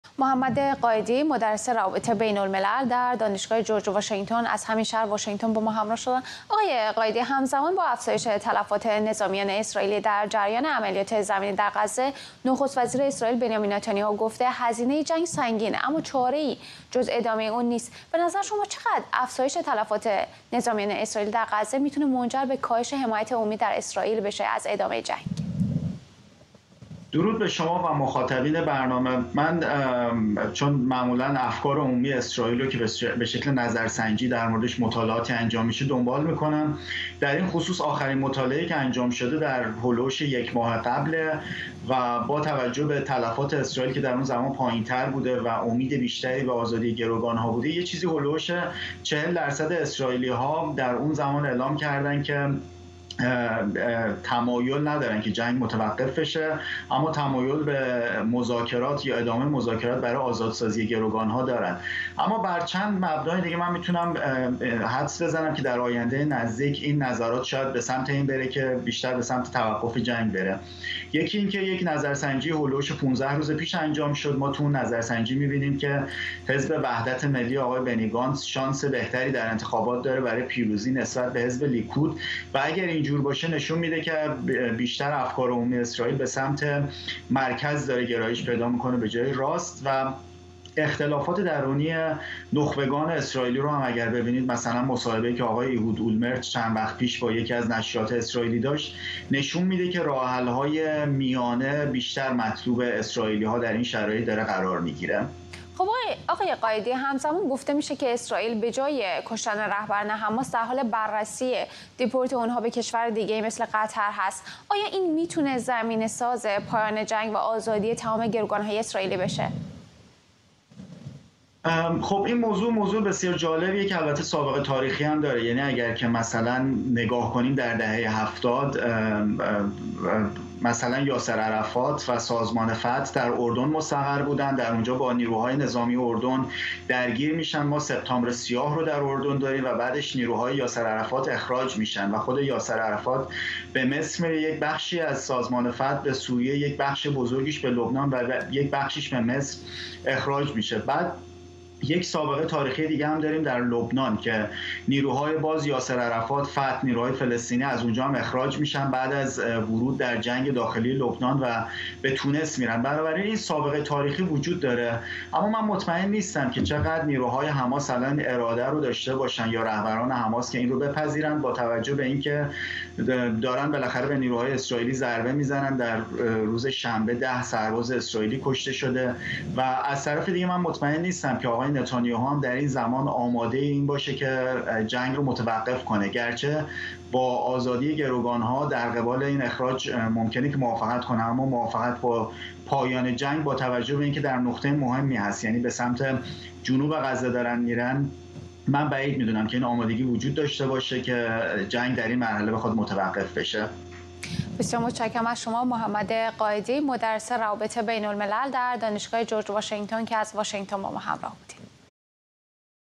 نتانیاهو: جنگ غزه هزینه بسیار سنگینی به اسرائیل تحمیل کرده است اما چاره‌ای جز ادامه نیست
نتانیاهو: جنگ غزه، هزینه بسیار سنگینی به اسرائیل تحمیل کرده است، اما چاره‌ای جز ادامه دادن نیست

گفت‌وگو با محمد قائدی، مدرس روابط بین‌الملل